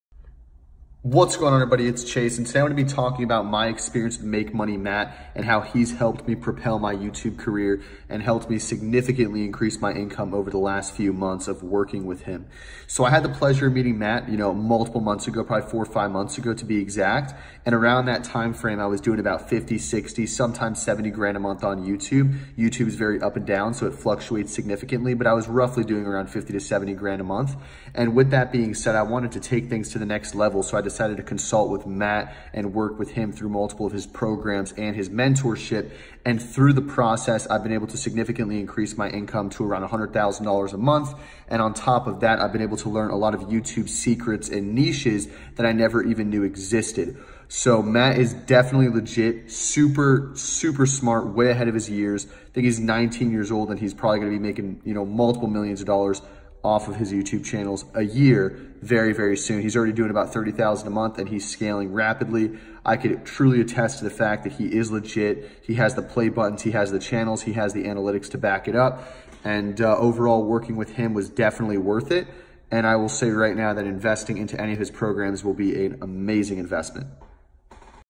Chase Namic Testimonial on Matt Par
Millionaire YouTube entrepreneur, Chase Namic, who earns $100,000 a month from YouTube revenue, has taken Matt's course and gives a flattering review.